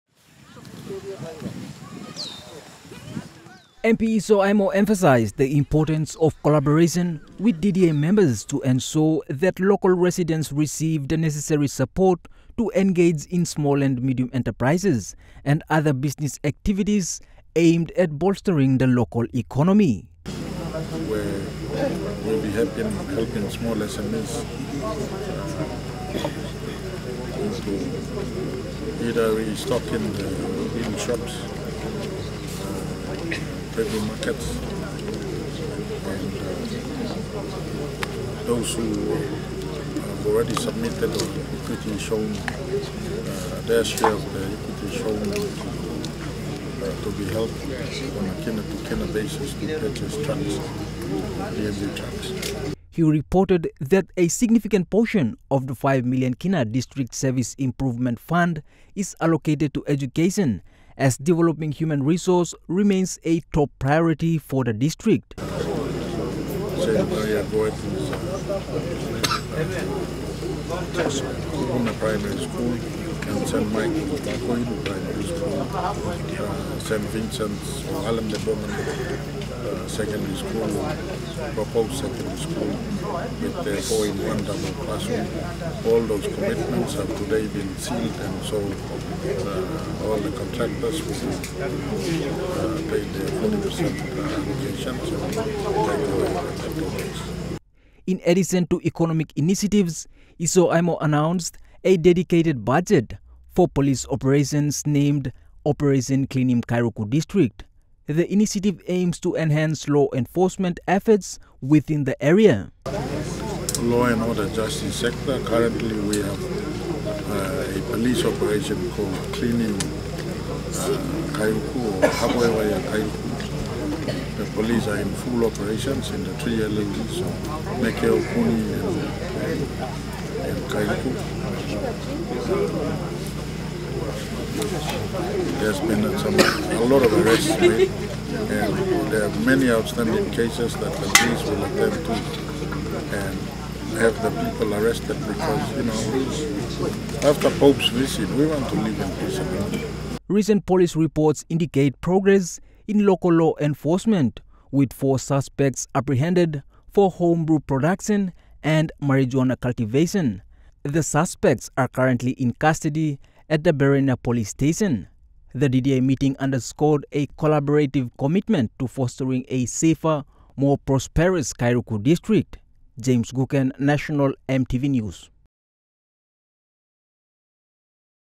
KAIRUKU DDA MEETING
The Kairuku District Development Authority (DDA) convened its third District Development Meeting yesterday at the Bereina Station within the DDA Admin block.